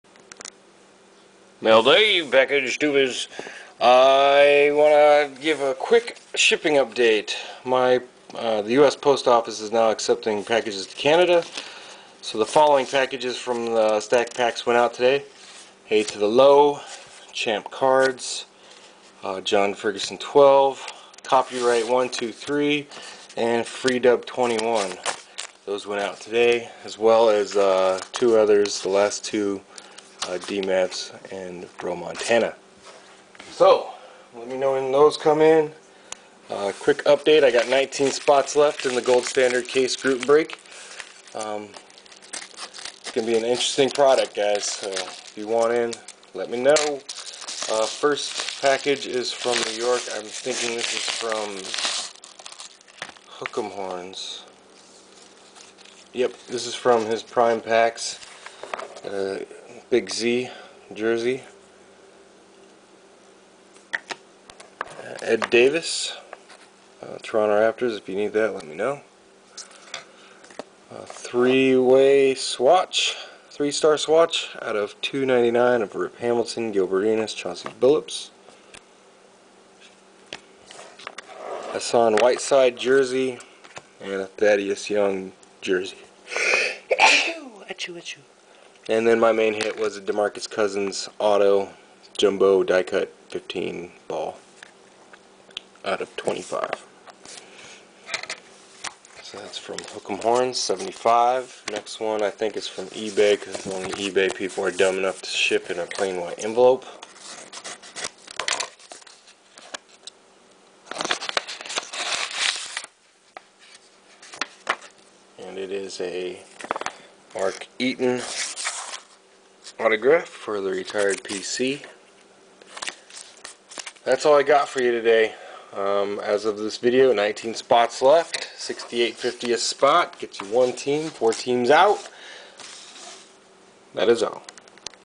Mailday (6/28/11) from Hookemhornz75 & eBay plus shipping update
All packages to Canada have been sent out today.  Please let me know when they arrive.